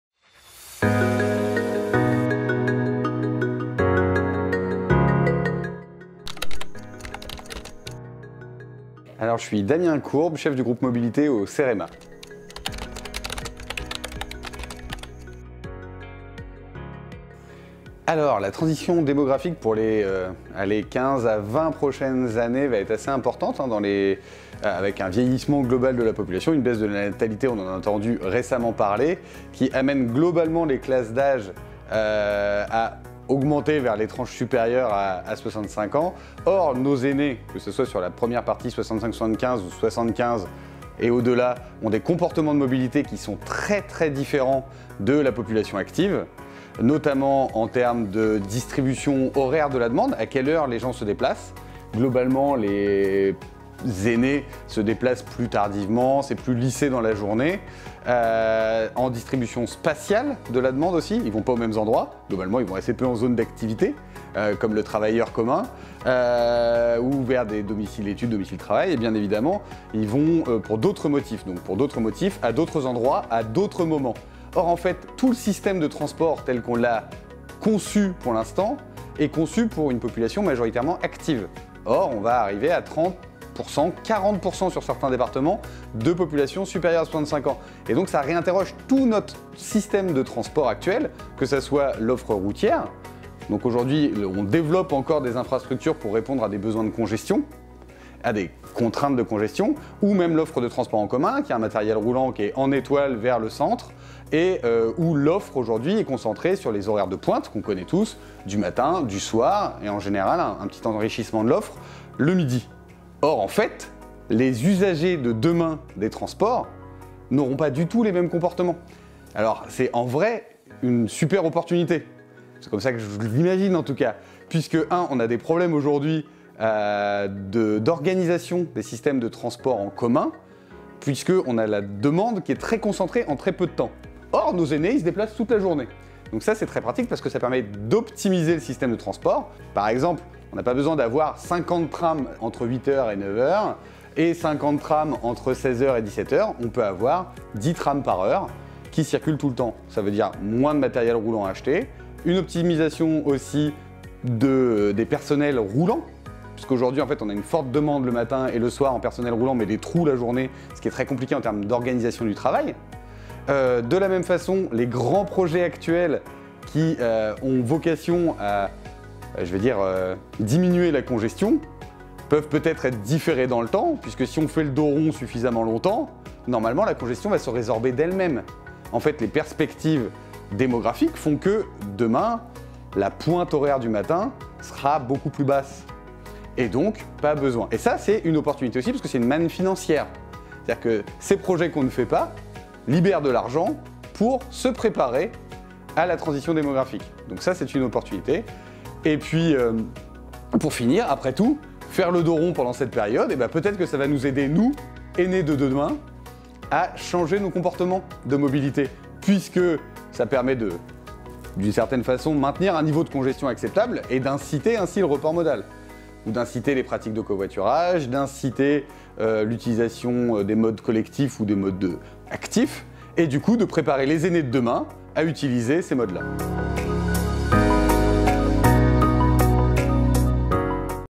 Colloque sur la mobilité des aînés : interview de Damien Courbe du Cerema
Découvrez l'interview de Damien Courbe du Cerema au sujet du lien entre la transition démographique et les politiques mobilités. Cette interview a été tournée lors de notre colloque mobilité des aînés qui a eu lieu au Centre de Congrès d'Angers, le 6 février 2024.

- Damien Courbe, chef du groupe mobilité au Cerema